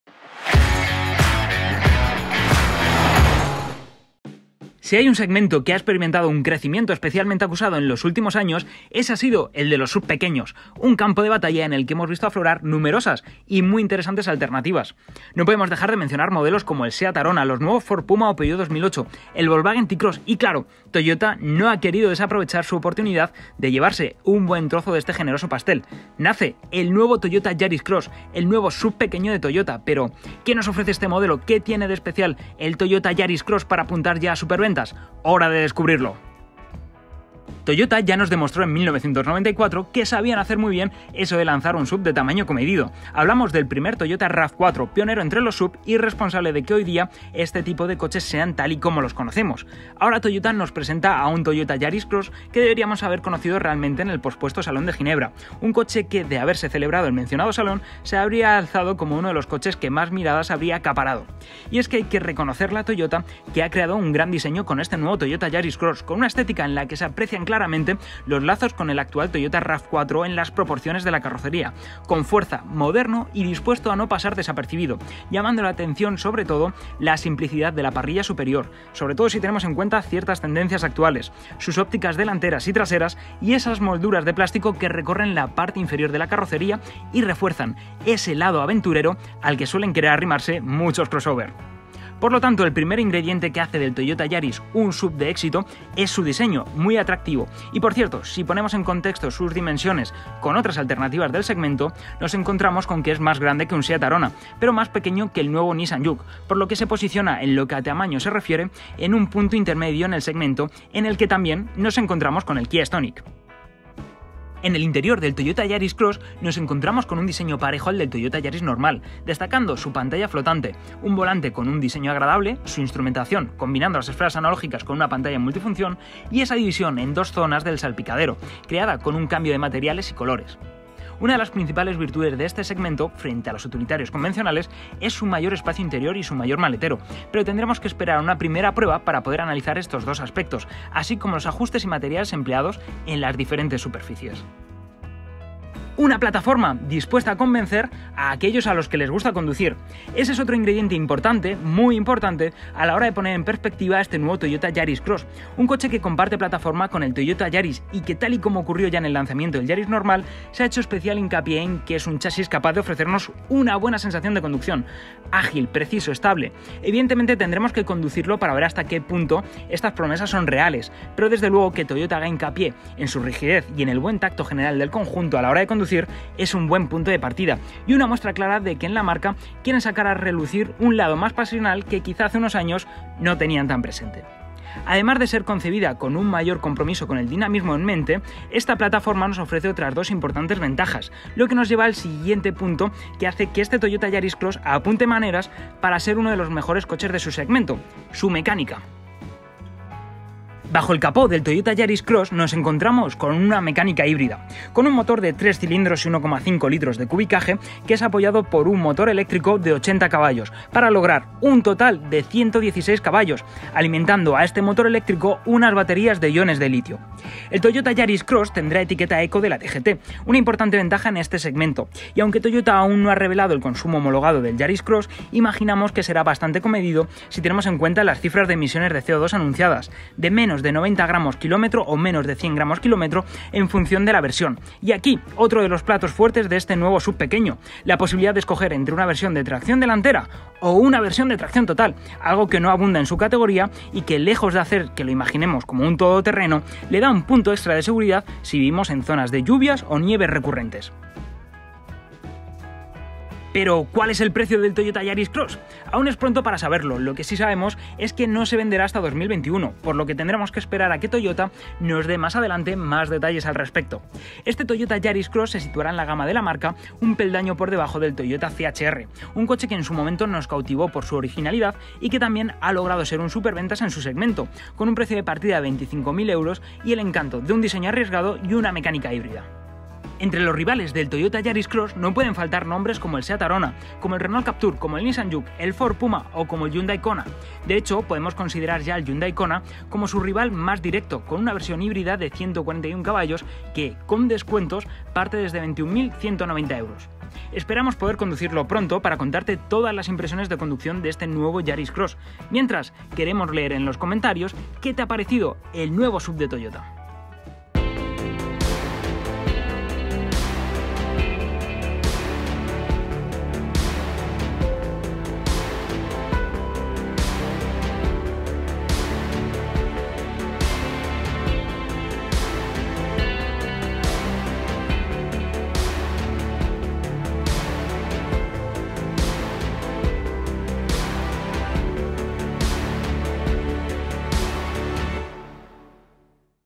Toyota Yaris Cross 2020 ¿el mejor SUV pequeño? | Análisis | Diariomotor
Toyota se adentra en el segmento de los SUV pequeños con el nuevo Toyota Yaris Cross, un SUV que se enfrentará a coches como el Hyundai Kona o el SEAT Arona y que analizamos en un nuevo vídeo para conocer todos los detalles que lo convierte en un serio aspirante a superventas.

Presentado por: Mario Herraiz (@herraizm)
Editado por: Laura Bastón (@laurabpazo)